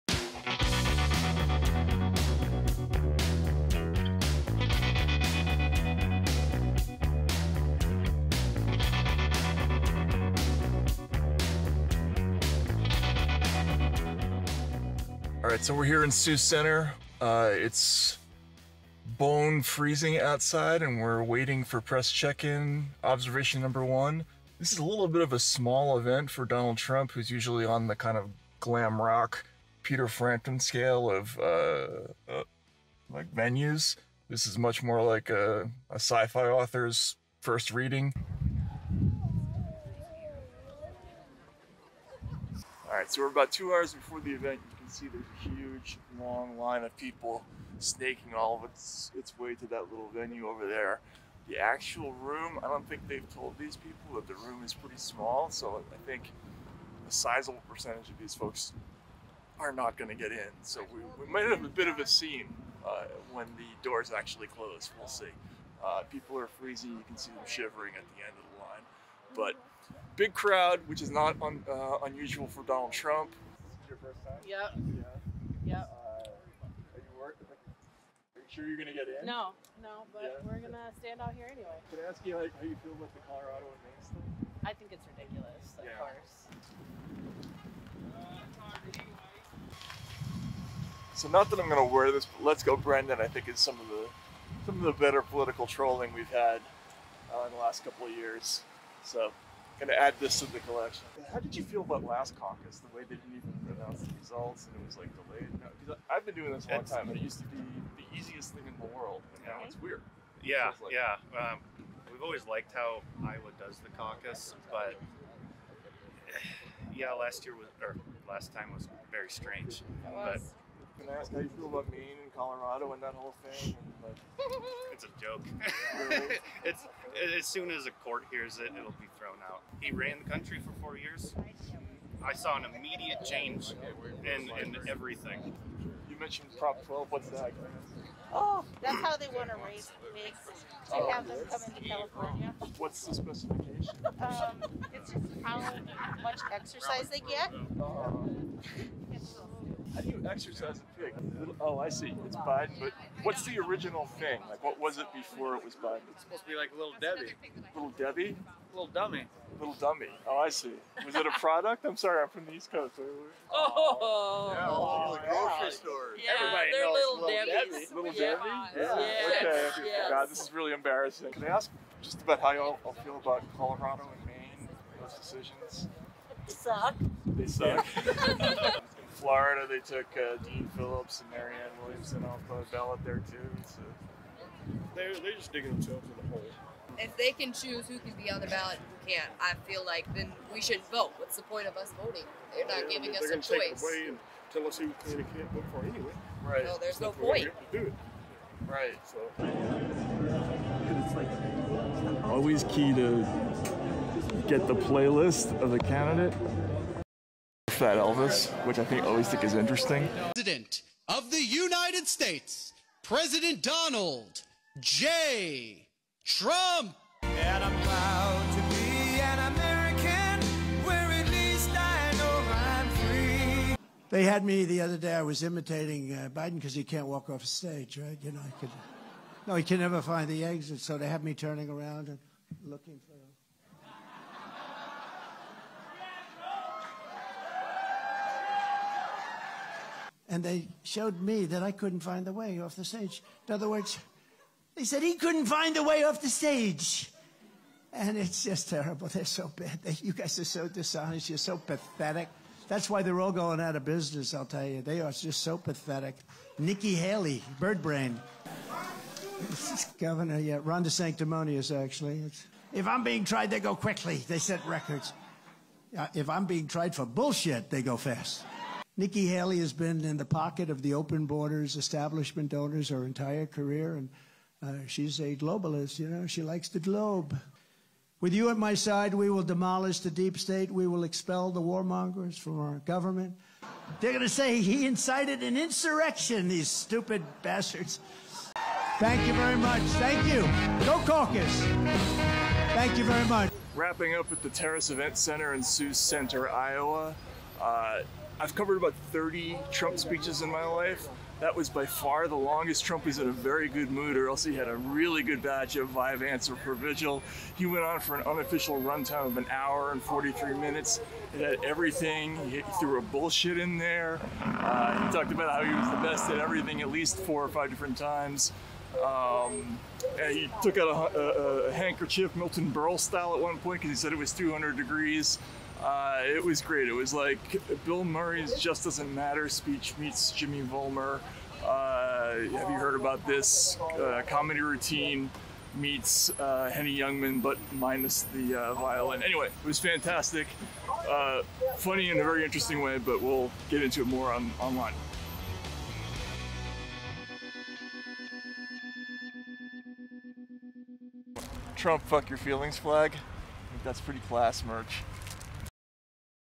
Matt Taibbi Visits Sioux Center Iowa's Commit to Caucus for Trump Rally
Trump speech footage from KTIV4